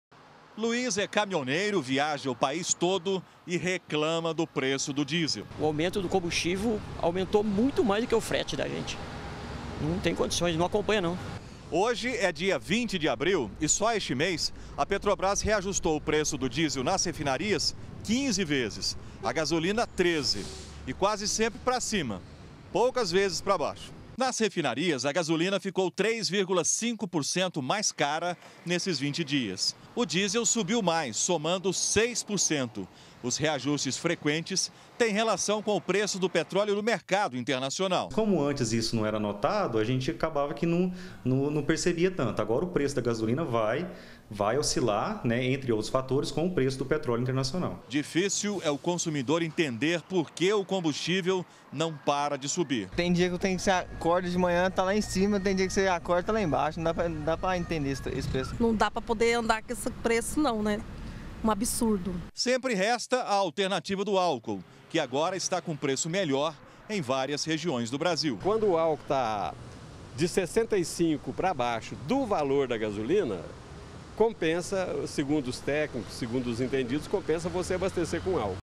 BG - Petrobras anuncia novo aumento - 21-04-2018
A Petrobras anunciou mais um aumento no preço dos combustíveis. A partir de hoje, a gasolina e o diesel vão subir pouco mais de um por cento, nas distribuidoras.